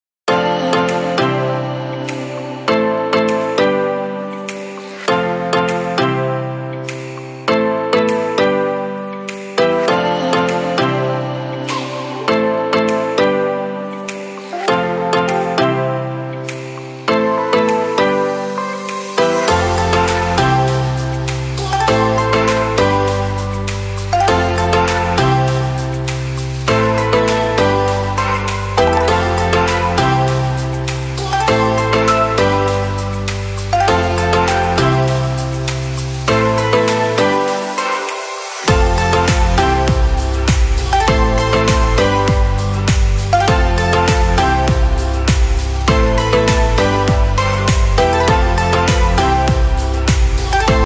It's impossible for a lazy person like me to clip socks one by one.
It's impossible for a lazy person like me to clip socks one by one. It's very convenient to use this drying net bag.